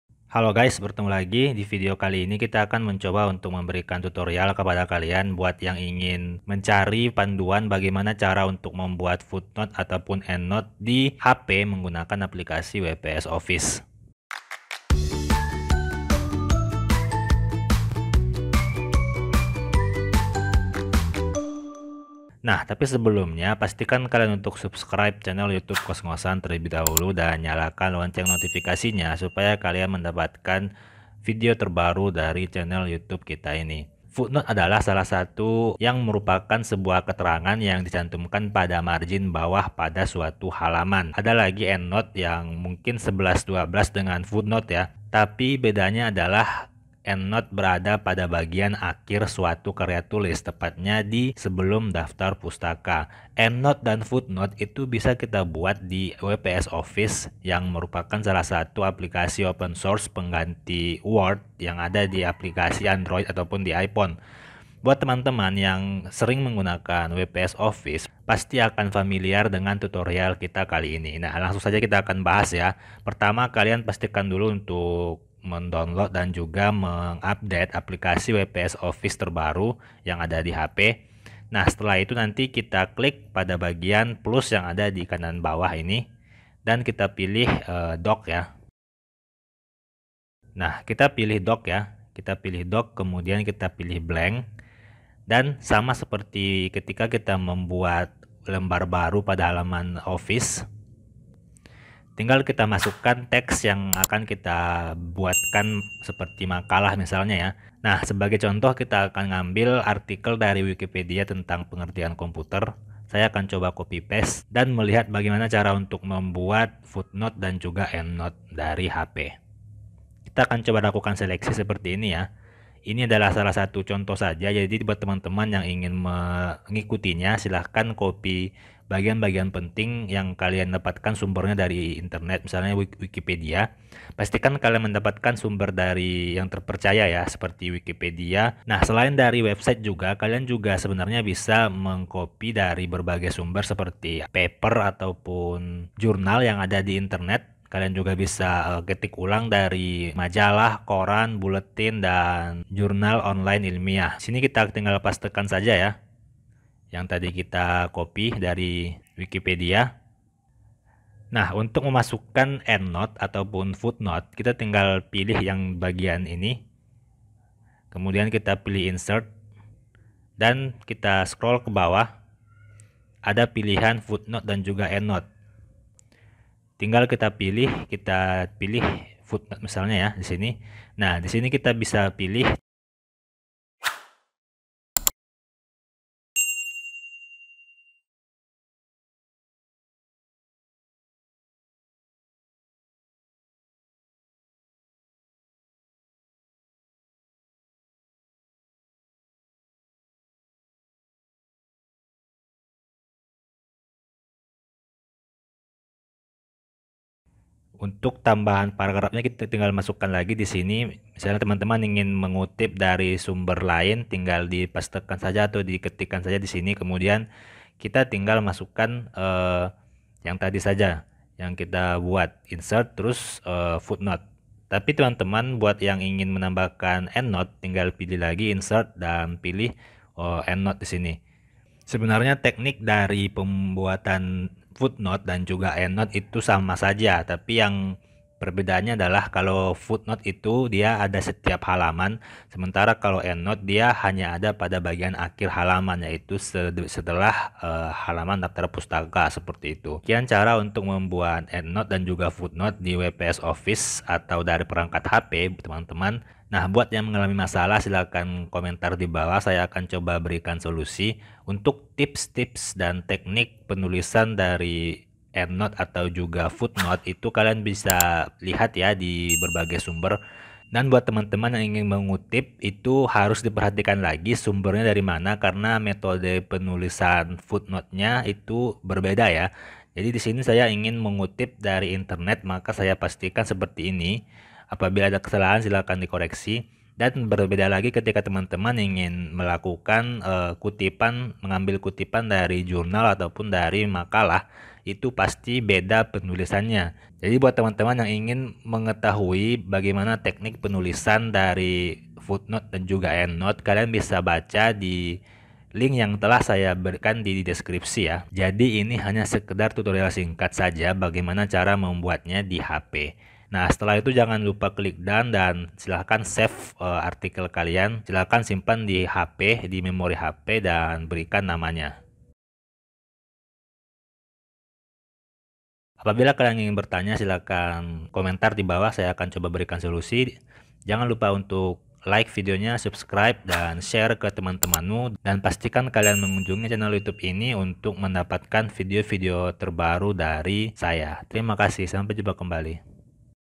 Cara Membuat FootNote dan EndNote di WPS Office HP
Cara membuat footnote dan endnote di WPS Office memakai HP Android dan Iphone dapat dengan mudah dilakukan. Video kali ini akan membantu sobat kosngosan dalam membuat catatan kaki memakai aplikasi WPS Office dan Office hp lainnya